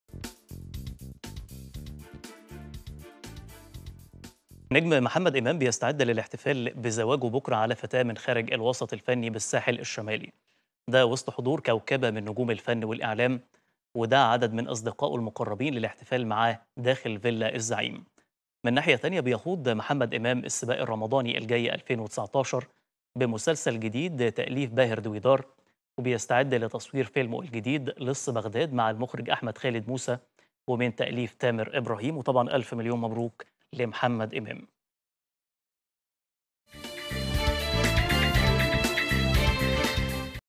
محمد إمام يحتفل بزفافه والعروسة مفاجأة
محمد إمام يحتفل بزفافه غداً والعروسة مفاجأة